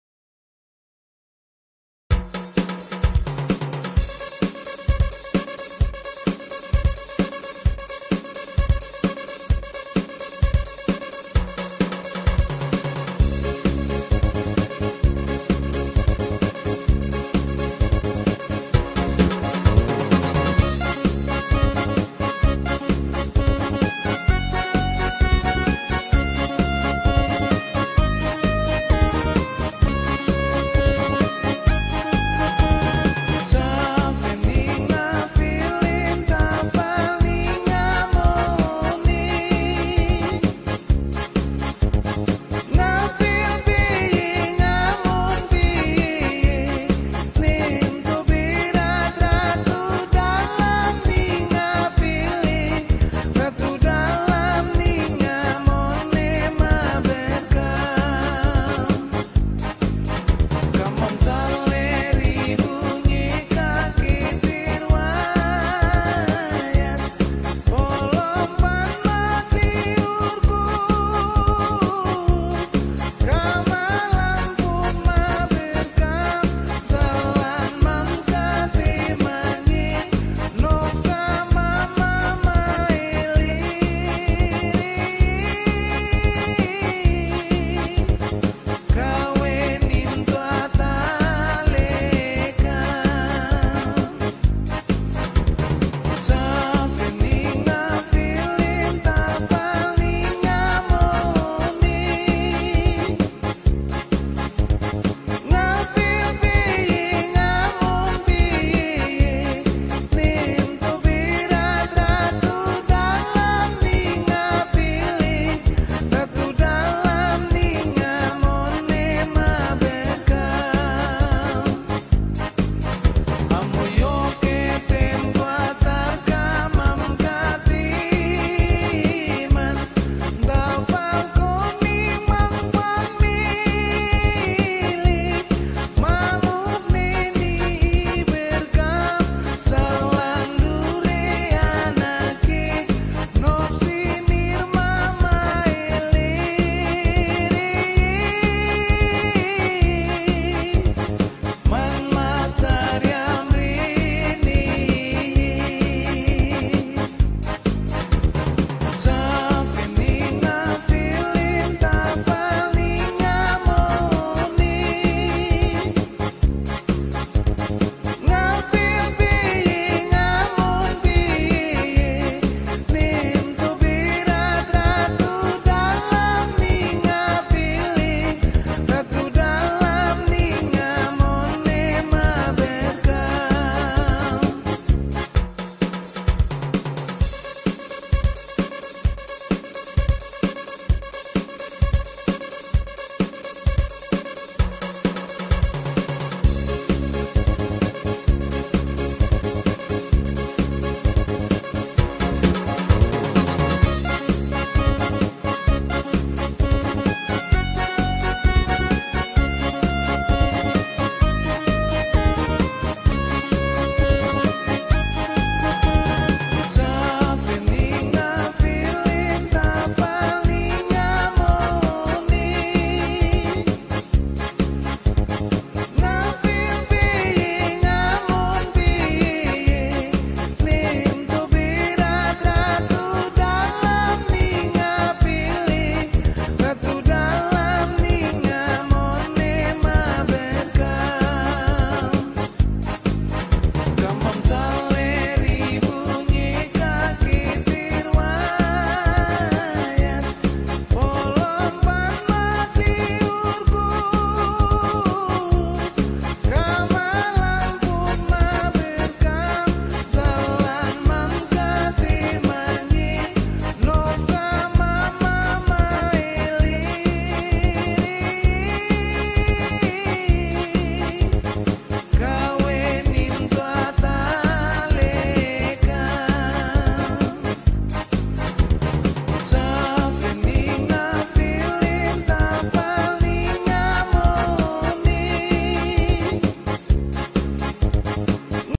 St Benediktus (Safe ni Ngafele)
Umat Rukun Santo Benediktus Mengantar Kristus Raja Semesta Alam ke Rukun Santo Agustinus (Paroki Ratu Rosario Suci Olilit Timur) Maluku indonesia